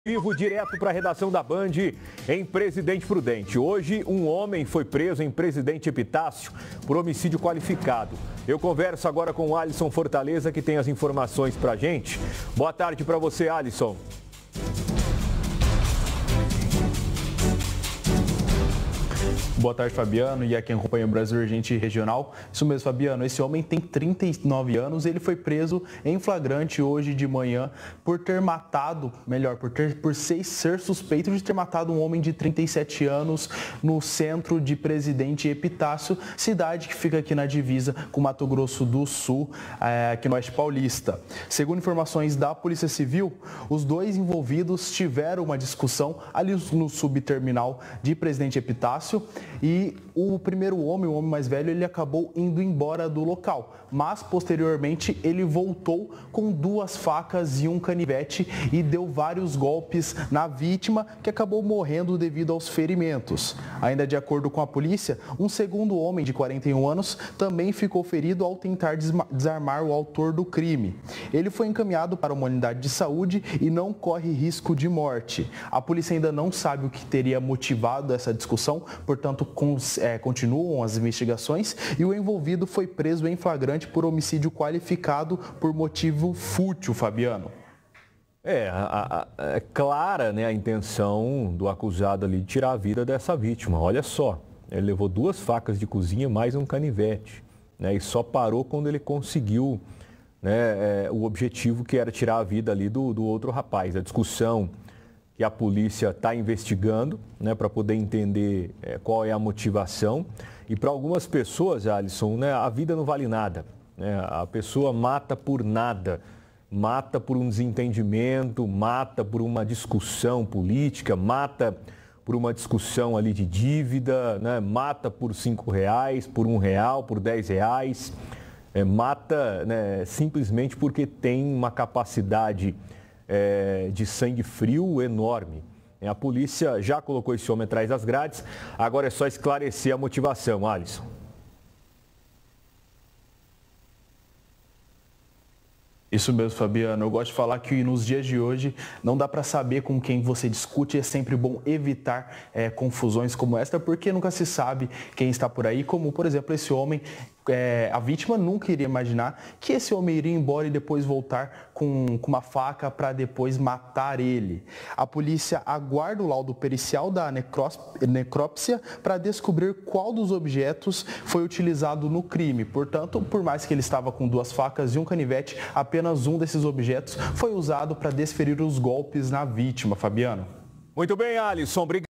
Homem é preso suspeito de homicídio em Presidente Epitácio
Um homem foi preso por homicídio qualificado em Presidente Epitácio (SP). De acordo com a polícia, a suspeita é que ele teria matado outro homem de 37 anos no centro da cidade, após uma discussão.